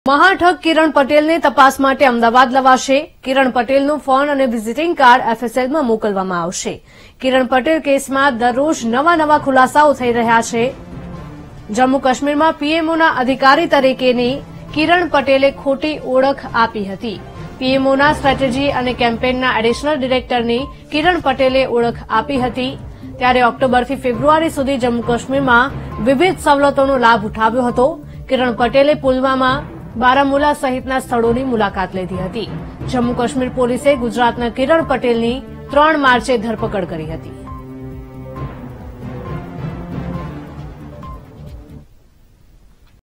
Ahmedabad News: મહાઠગ કિરણ પટેલને તપાસ માટે અમદાવાદ લવાશે
Ahmedabad News: કિરણ પટેલનો ફોન-વીઝીટીંગ કાર્ડ FSLમાં મોકલાશે, કિરણ પટેલ કેસમાં દરરોજ નવા નવા ખુલાસા થઈ રહ્યા છે | VTV GUJARATI